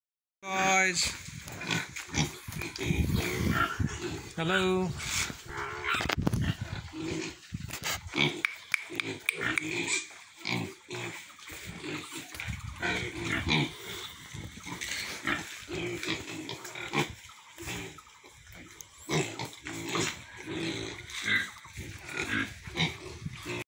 बङ्गुरका पाठापाठि
यो भिडियो एग्री प्योर प्रा लि चक्रघट्टी सुनसरी मा लिएको हो । यहाँ भएको रातो रङ्गको पाठीको जात ड्युरोक हो भने अन्य ह्याम्पसायर जातको हो ।